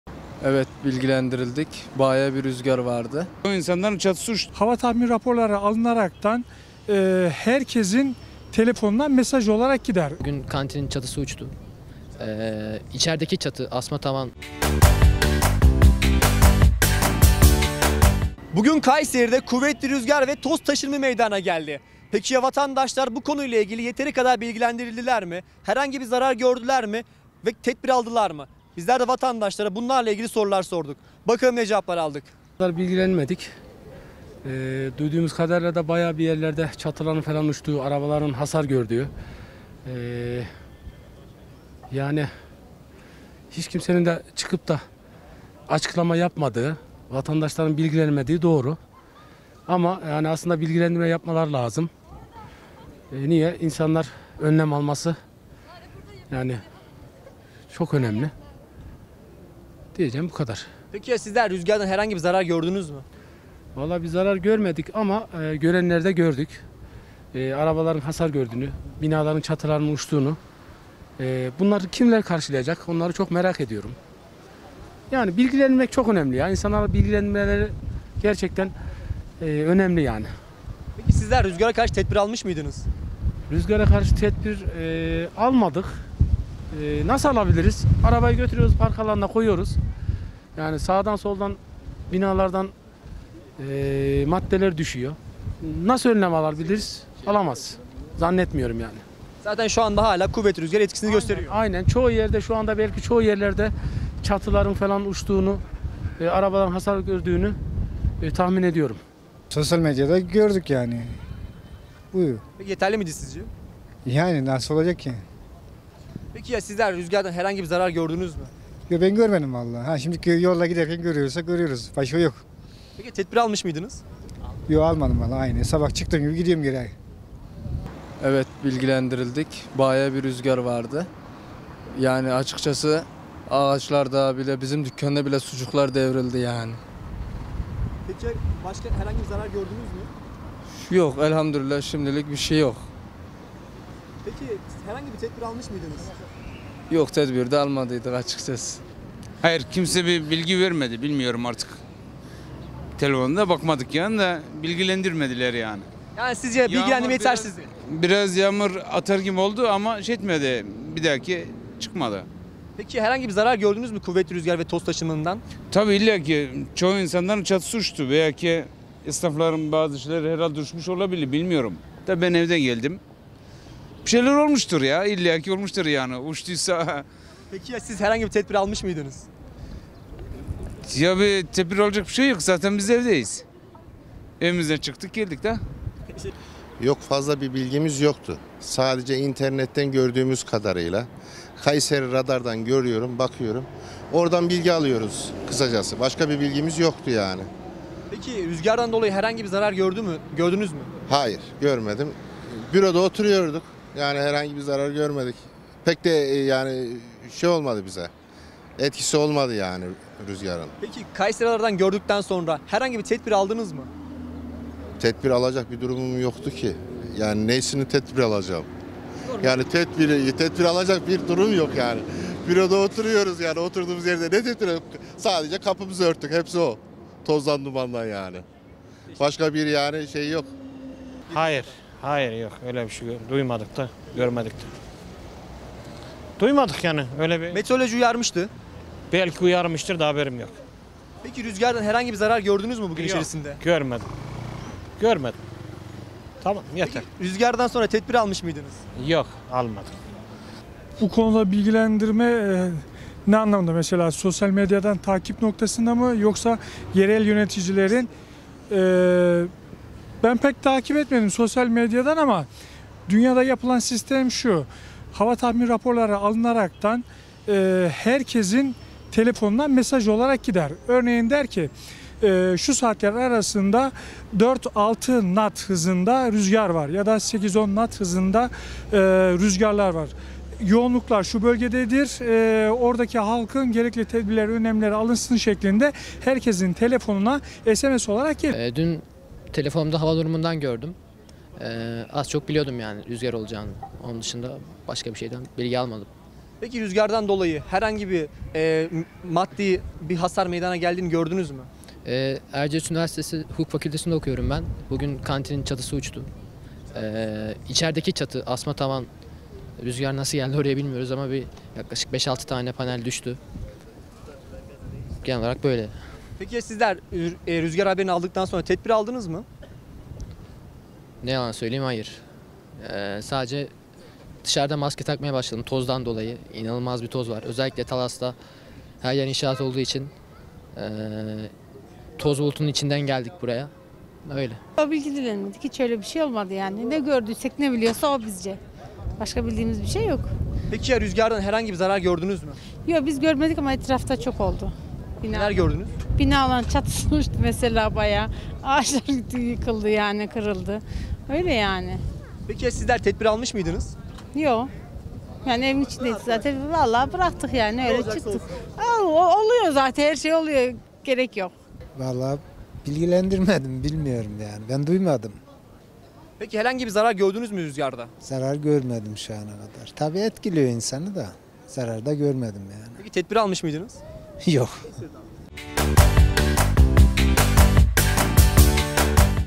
Kayseri'de Şiddetli Rüzgar
Kayseri genelinde etkili olan fırtına nedeniyle kentin değişik noktalarında ağaçlar devrildi, evlerin çatıları uçtu. Araçlar başta olmak üzere büyük çapta maddi hasar oluştu.
Kayserimnet ekibi "Kent genelinde etkili olan şiddetli rüzgar ve toz aşımında ne gibi tedbirler aldınız" diye sordu.